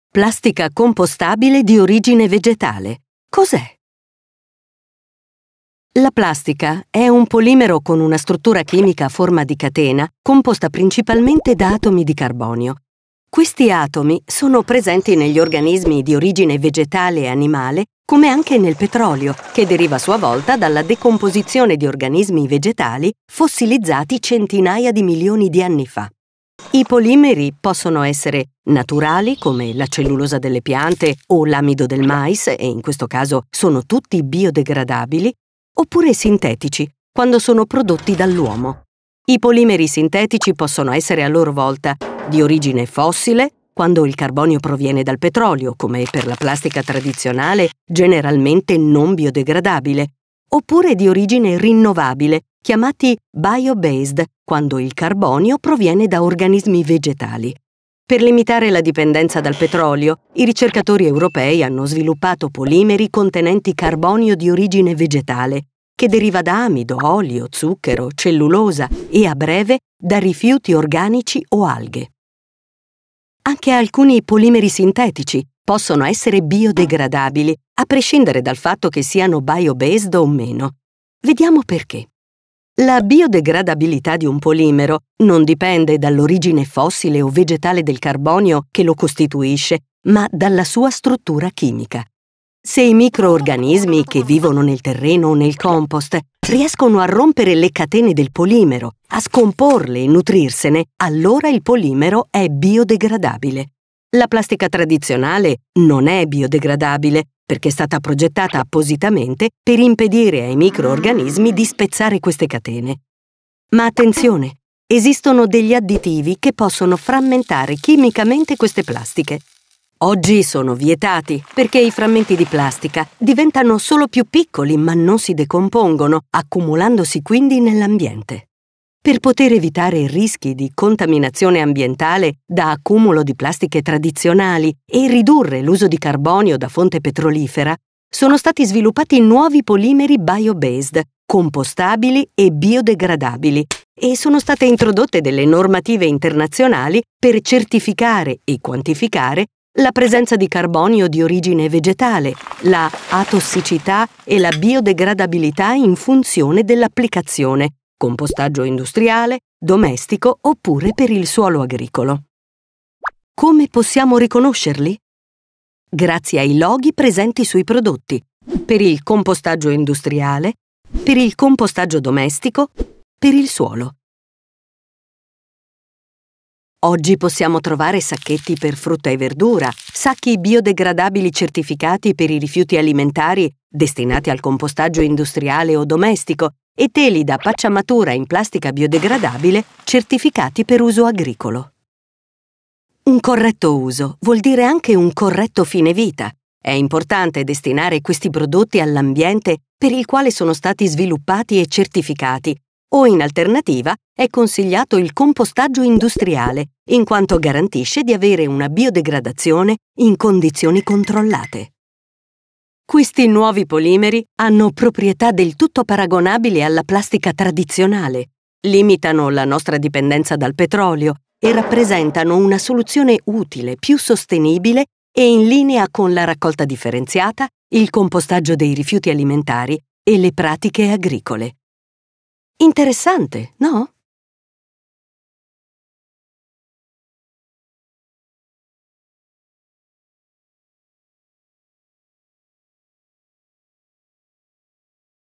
Cosa si intende per plastica compostabile di orgine vegetale?
Tra i numerosissimi polimeri che la chimica sa oggi produrre, vi sono anche i polimeri biodegradabili di origine vegetale. In questo video potrai approfondire la loro composizione, perché e come si biodegradano.